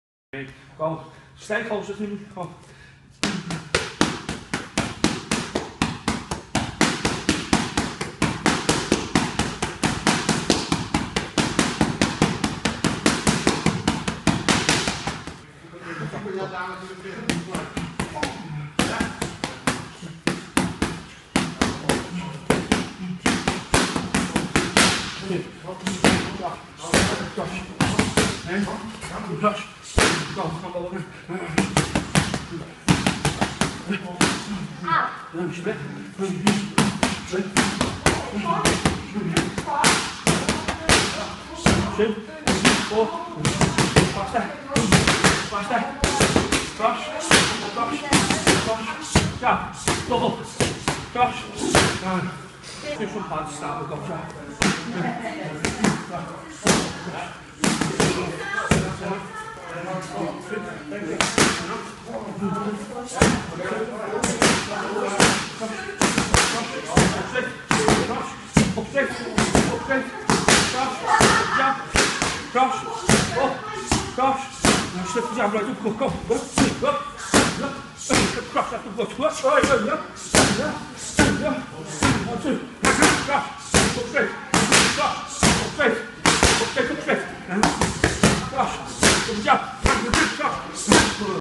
Sammy Preston at Masda Gym
Sammy Preston doing some boxing pads with Coach Alex Forman